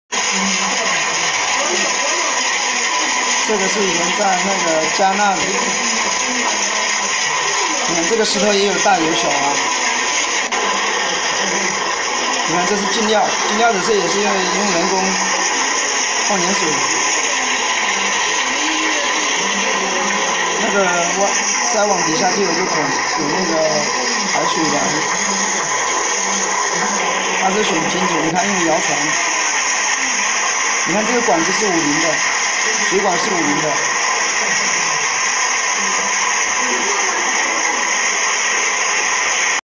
Roller washing machine and vibrating screen using video
The rotary scrubber is an autogenous rotating drum scrubber, utilizing pebbles and rocks within the feed source to fracture and disintegrate clay, agglomerates and soft oxides. The scrubber spins slowly, similar to a giant washing machine. It churns the fines and oversize and water together, vigorously breaking up the soil matrix as well as liberating the target mineral from tough clay, schist’s and rock.